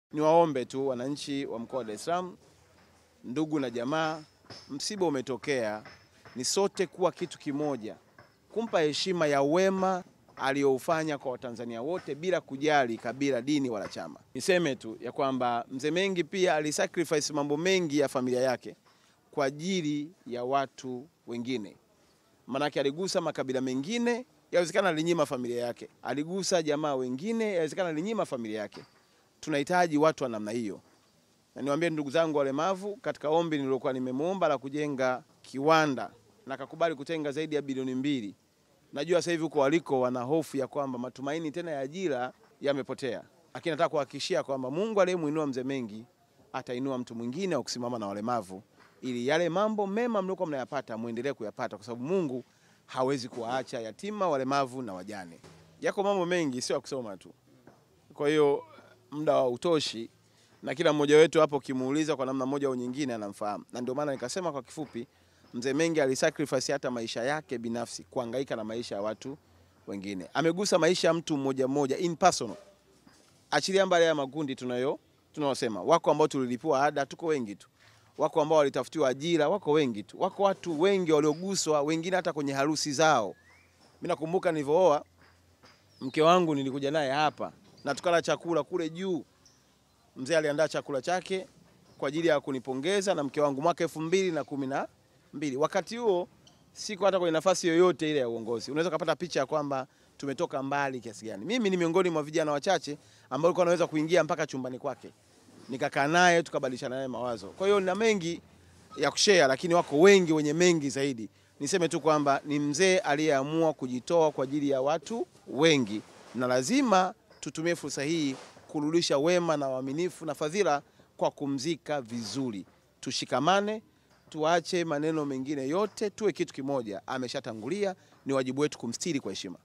KAULI YA MKUU WA MKOA WA DAR ES SALAAM MENGI ALIKUBALI KUTENGA BILIONI 2
RIP MENGI TASNIA ITAKUKUMBUKA KWA UWEKEZAJI WAKO MKUBWA KWENYE TASNIA YA HABARI#mleletvonline